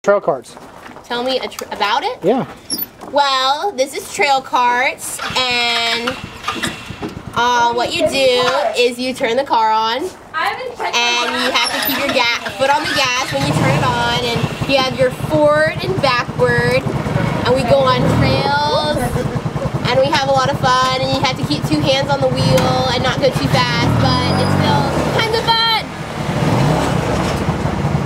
Camp Mac Trail Carts
Camp Mac Trail Carts. Girl Campers describes the trail carts elective.